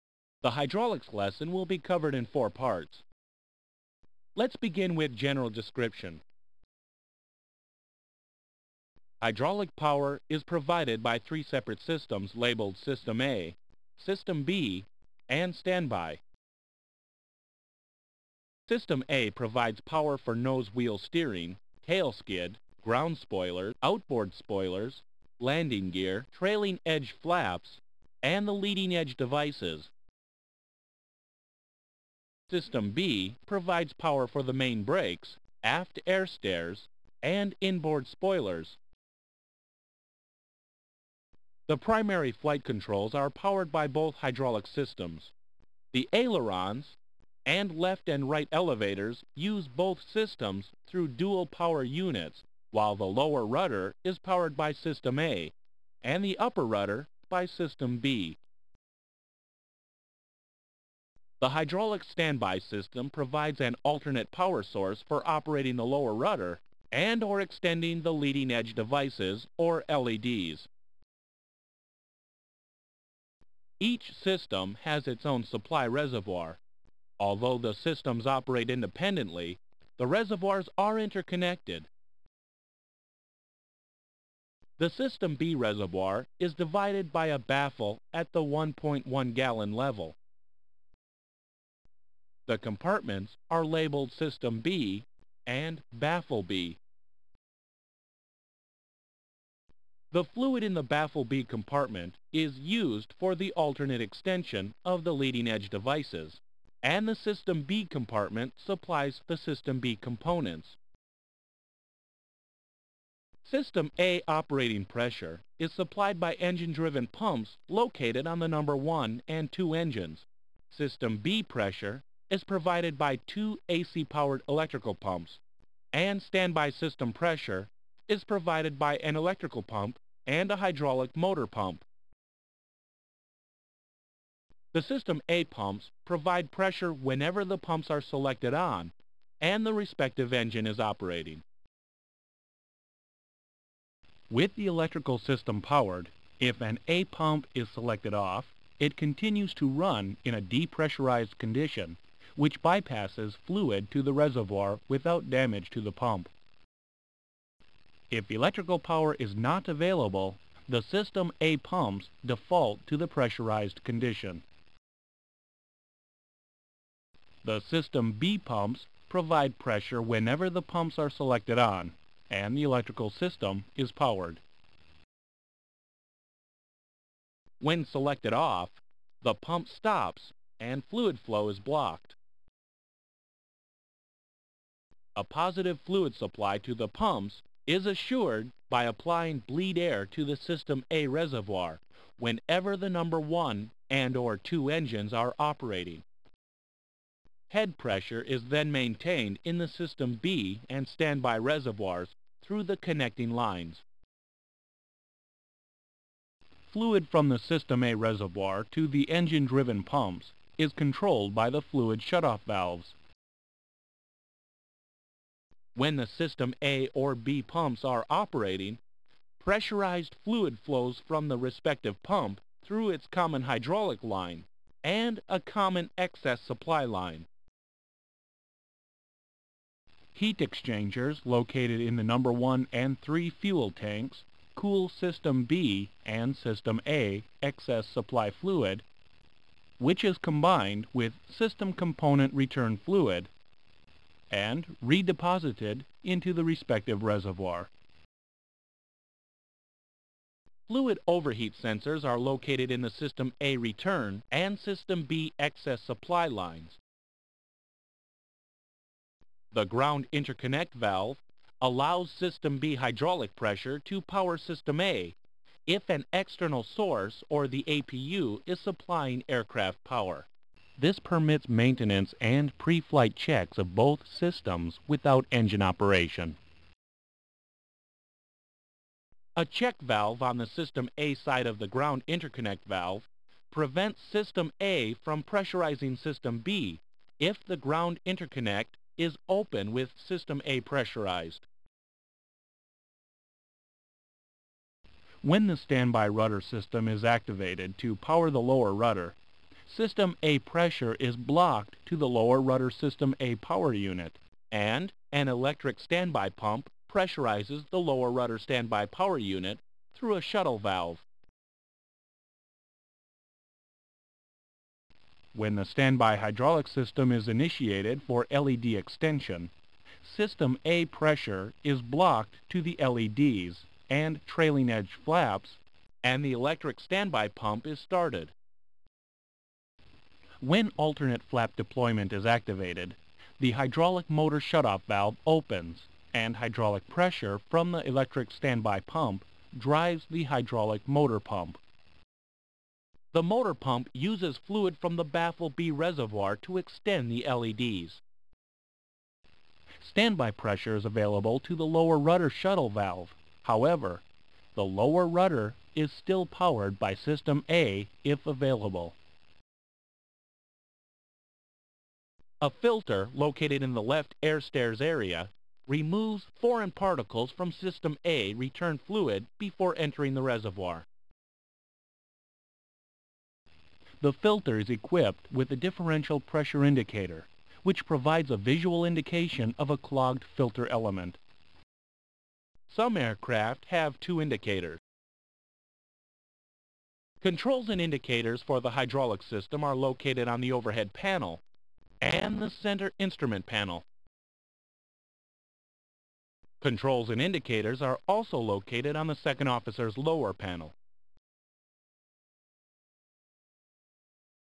B727 Hydraulics - General Description
System consists of a single reservoir . Hydraulic power is supplied to the A system from two engine driven pumps fitted to engines No1 and No2 . The pump switches control solenoid operated blocking valves, with the switches on, normal pump output is supplied to the system. A small portion of the fluid that enters the pump is circulated through the pump case for cooling and lubrication. As long as the engines are operating regardless of the blocking valve position this small amount of fluid leaves the pump and is cooled by a heat exchanger in the No3 fuel tank heat exchanger before being returned to the system.
fluid shutoff valves controlled by there respective switches control the fluid in the engine pump supply lines, when closed fluid flow to the pump is isolated. (includes lubricating and cooling fluid). pulling the fire switch on engine 1 or 2 will close these shutoff valves, the fire switch also deactivates the low pressure warning light.
System A and therefore system B and stand by reservoirs are pressurized by engine bleed air from engines 1 & 2 this is applied to the reservoir to ensure a positive supply of fluid to the pumps.
A balance line connects the system A reservoir to the system B reservoir at a level of 2.5 gall.